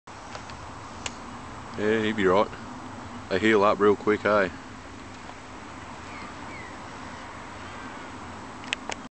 Evil rabbs good sized redfin
Old rabbit caught a good sized redfin on his first trip on LBG in the tinny.I think hes hooked now.